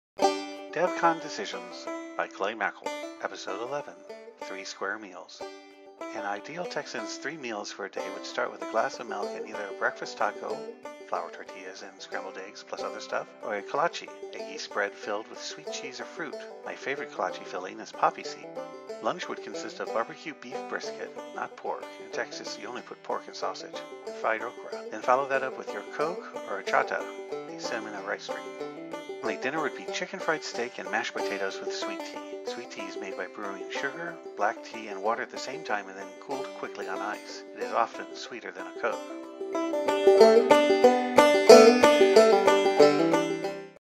FileMaker DevCon 2014 Tips: Three Square Meals Texas Style!
Everything is bigger in Texas - including having three square meals! In this clip, Clay Maeckel, Chief Software Architect, FileMaker shows us what those look like and includes some tips on treats visitors should try before leaving San Antonio.

Want more information about this year's FileMaker Developer Conference?